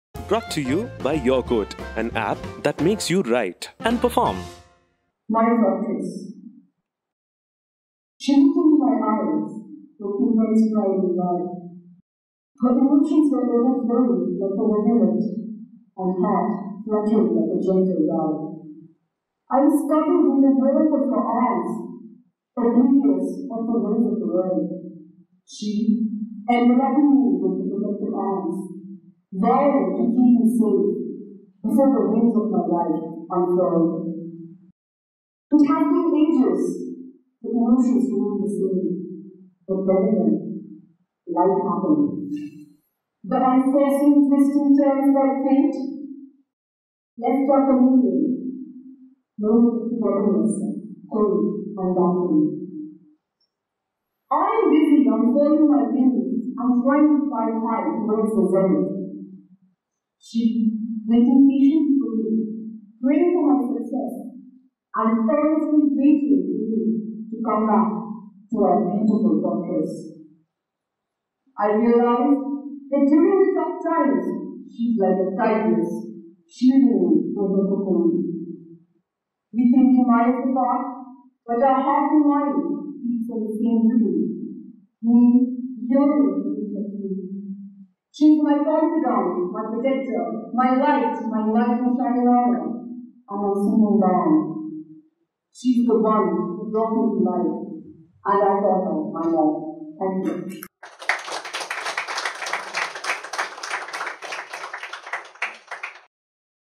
'My Worth Is' by Anurupa Ghosh | English Poetry | YQ - IMT Nagpur (Open Mic 1)
Venue: LRC Hall, IMT Nagpur